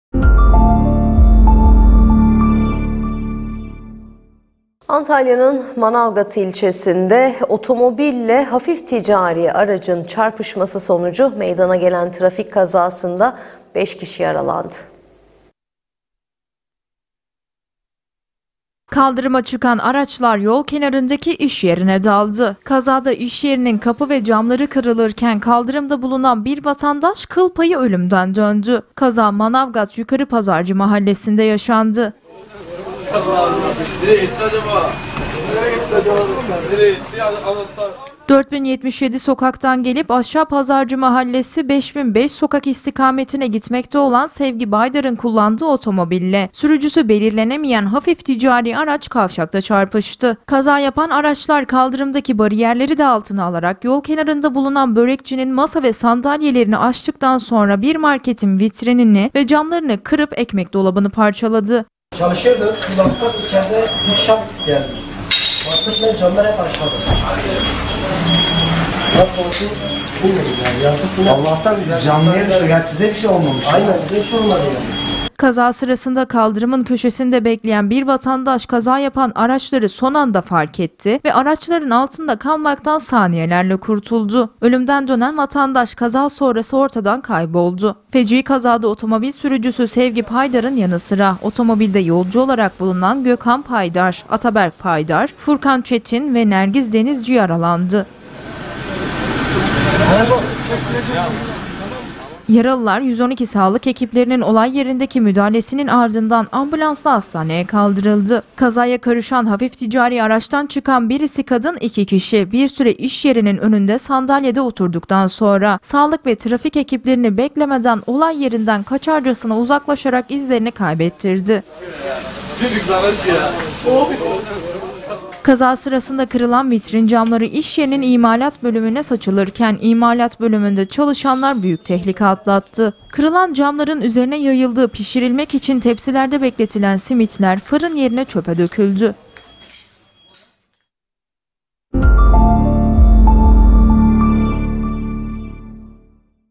KAZA YAPAN ARAÇLAR İŞ YERİNE GİRDİ
ANTALYA'NIN MANAVGAT İLÇESİNDE OTOMOBİL İLE HAFİF TİCARİ ARACIN ÇARPIŞMASI SONUCU MEYDANA GELEN KAZADA 5 KİŞİ YARALANDI. KAZA DA YARALILARA MÜDAHALE ETMESİ GEREKEN DİĞER ARAÇTAKİLER, ARKALARINA BİLE BAKMADAN OLAY YERİNDEN KAÇTI. KAZA SIRASINDA KALDIRIMDA BULUNAN VE ARAÇLARI SON ANDA FARK EDEN BİR VATANDAŞTA, KIL KIL PAYI ÖLÜMDEN DÖNDÜ.